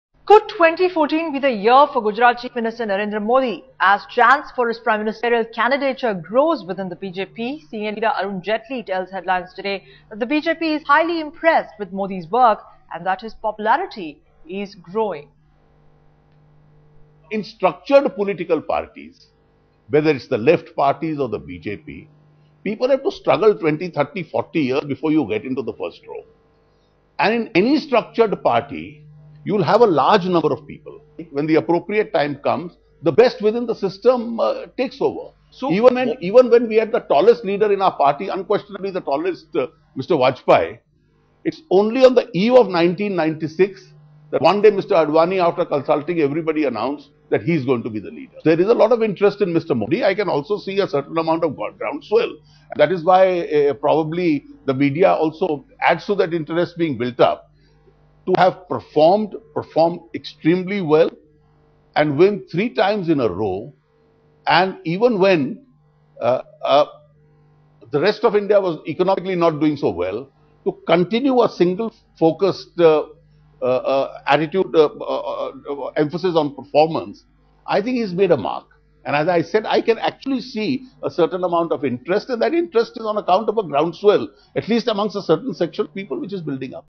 Growing popularity for Modi within BJP, says Jaitley
Senior BJP leader Arun Jaitley admits that the party is highly impressed with Narendra Modis work and that he has made a mark. It is evident that the chance for Narendra Modi PM candidature is growing within the BJP.